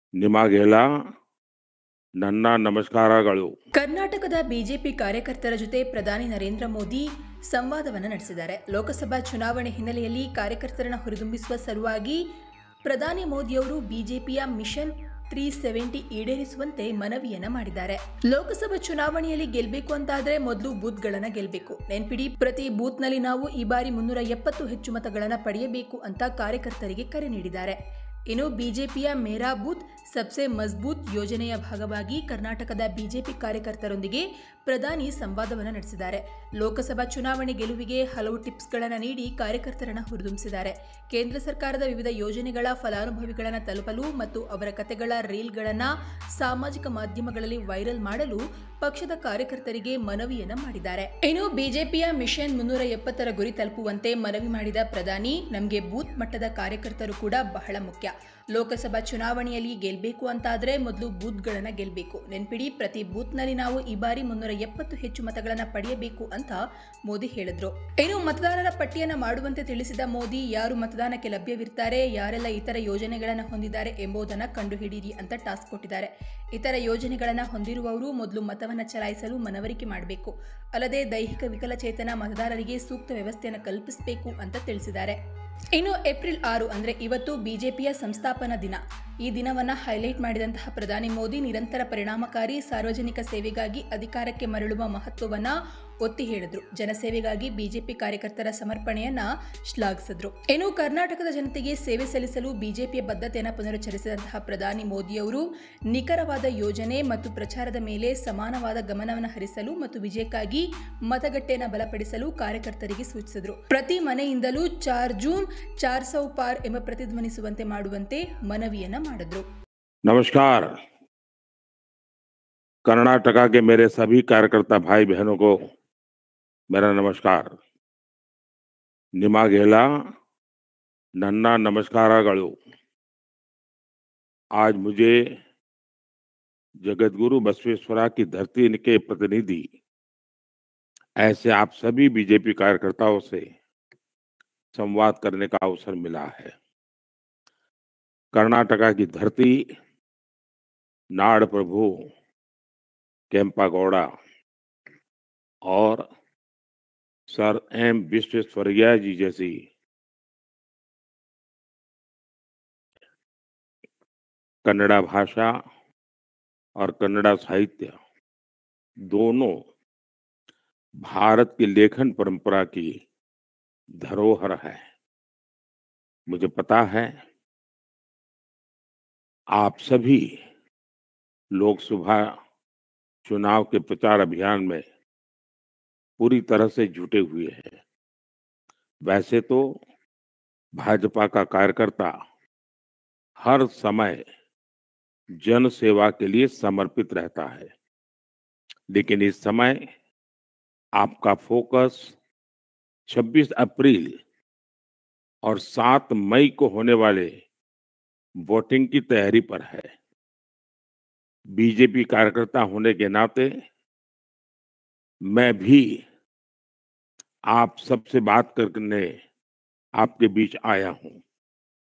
Karnatakaದ BJP ಕಾರ್ಯಕರ್ತರ ಜೊತೆ ಪ್ರಧಾನಿ ನರೇಂದ್ರ ಮೋದಿ ಸಂವಾದ!
ಕರ್ನಾಟಕದ ಬಿಜೆಪಿ ಕಾರ್ಯಕರ್ತರ (BJP workers) ಜೊತೆ ಪ್ರಧಾನಿ ನರೇಂದ್ರ ಮೋದಿ (Prime Minister Narendra Modi) ಇಂದು ಸಂವಾದ (interact) ನಡೆಸಿದರು. ಲೋಕಸಭಾ ಚುನಾವಣೆ (Lok Sabha Election) ಹಿನ್ನೆಲೆಯಲ್ಲಿ ಕಾರ್ಯಕರ್ತರನ್ನು ಹುರಿದುಂಬಿಸುವ ಸಲುವಾಗಿ ಸಂವಾದ ನಡೆಸಿದ ಪ್ರಧಾನಿ, ಬಿಜೆಪಿಯ 'ಮಿಷನ್ 370' (Mission 370) ಈಡೇರಿಸುವಂತೆ ಮನವಿ ಮಾಡಿದ್ರು.Prime Minister Narendra Modi interacted with Karnataka BJP workers today. In order to cheer up the activists in the background of the Lok Sabha Election, the Prime Minister requested them to fulfill BJP's 'Mission 370'